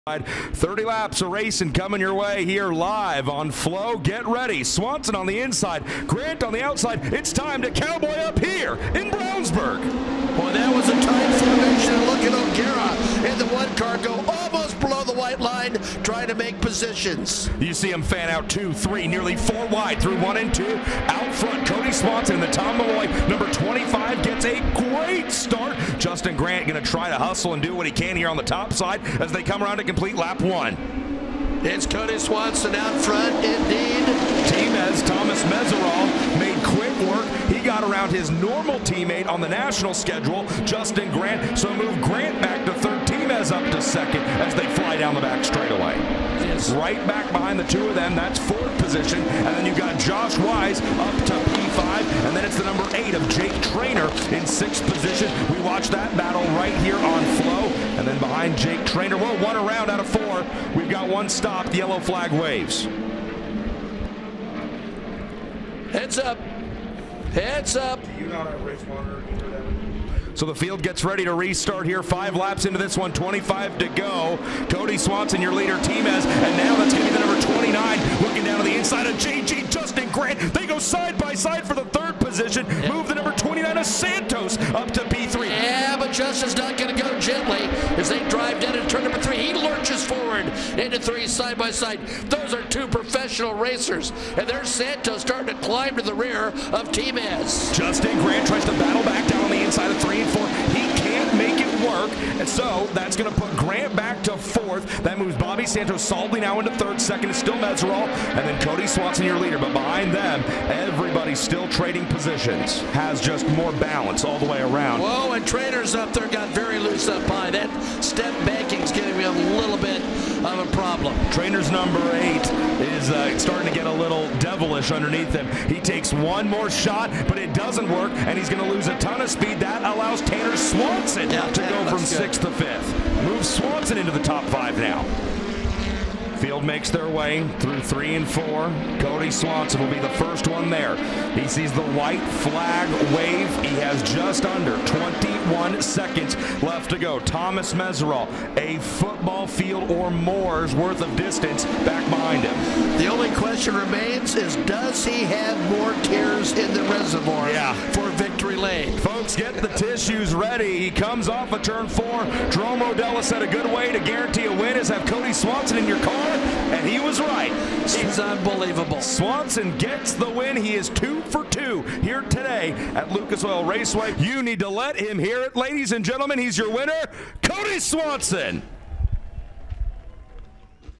Highlights: USAC Midget (SE) at Lucas Oil Raceway - May 29, 2021
ICYMI: Kody Swanson had a highly successful early Saturday, following up his victory in the USAC Silver Crown opener with another score in a dominating performance with the midgets to complete a Carb Night Classic sweep at The Oval at Lucas Oil Raceway in Brownsburg, Indiana.

Watch the full race on FloRacing!